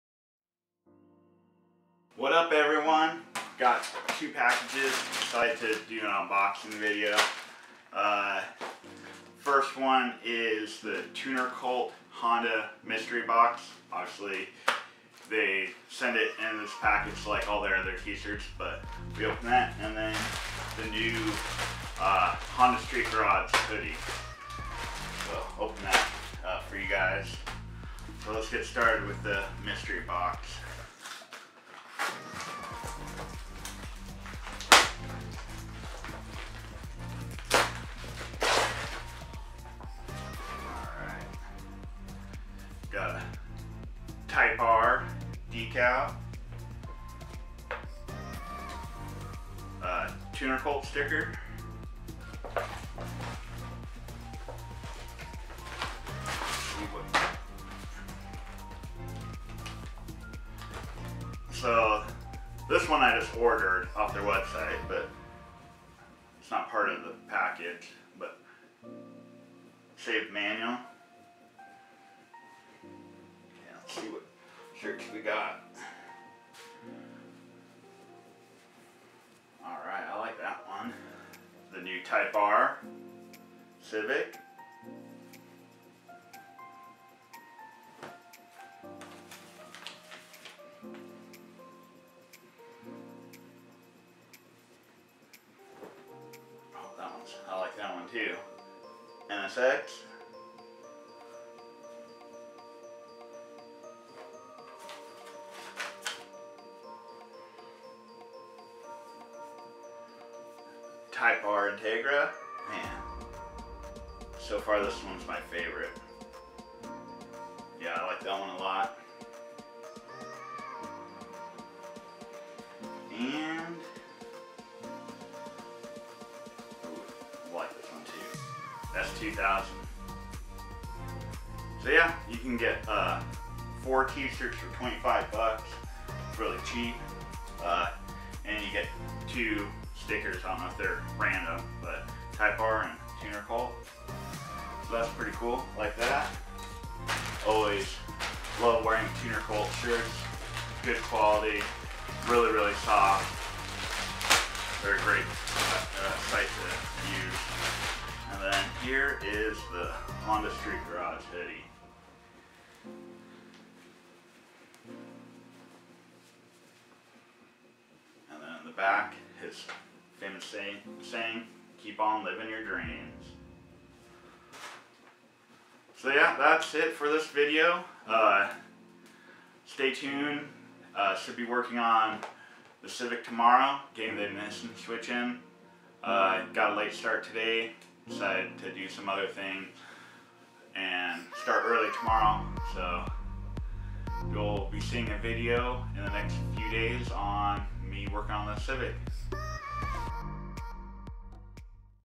Unboxing Tuner Cult Honda Mystery Box
Received my Tuner Cult & Honda Street Garage package in the mail today. Just a little unboxing video to see what was in the Honda mystery box.